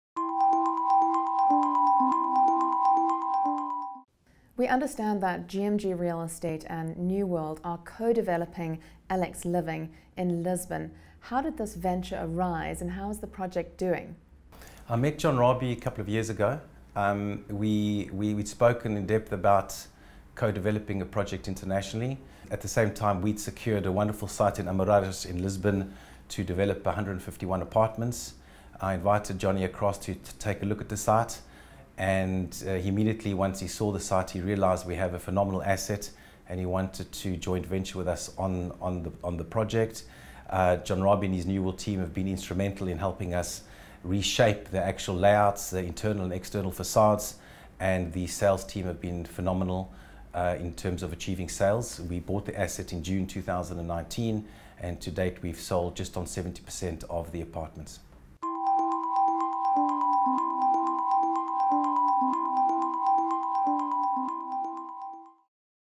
GMG Real Estate - LX Living - BLACK ONYX
BLACK ONYX interviews specialist asset managers. Here we discuss international property development LX Living with Newman Leech from GMG.

for the FULL INTERVIEW and earn CPD points from this type of content of THE CPD HUB